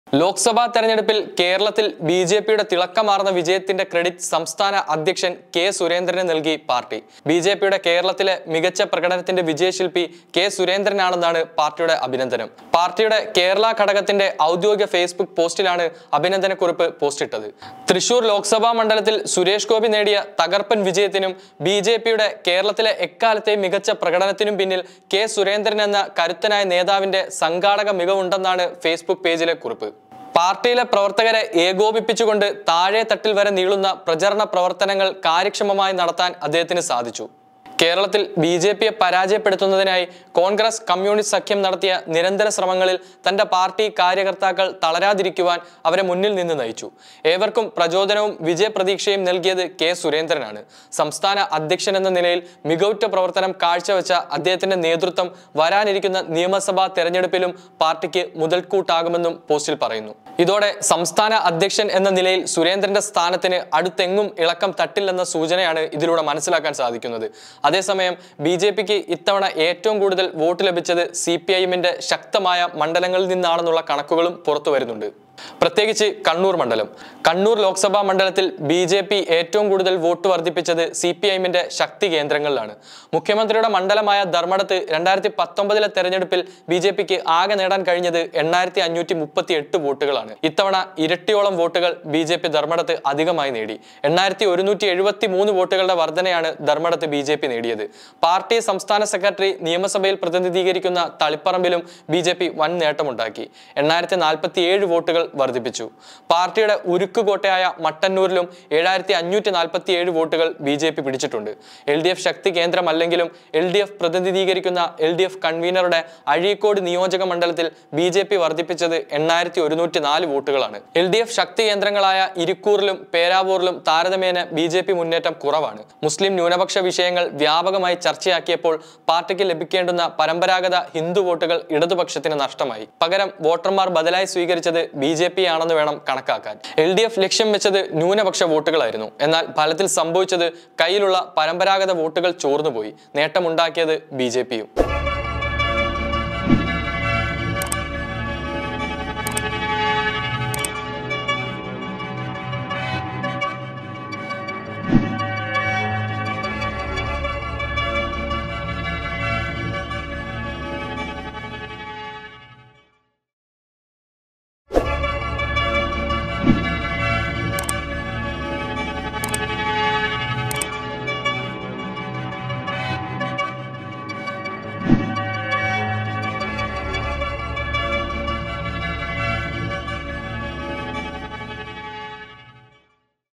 എല്ലാ ക്രെഡിറ്റും സുരേന്ദ്രന് …ബിജെപി ഏറ്റവും കൂടുതൽ വോട്ട് നേടിയത് ധർമടത്ത്‌ | Sark Live
എല്ലാ ക്രെഡിറ്റും സുരേന്ദ്രന് …

ബിജെപി ഏറ്റവും കൂടുതൽ വോട്ട് നേടിയത് ധർമടത്ത്‌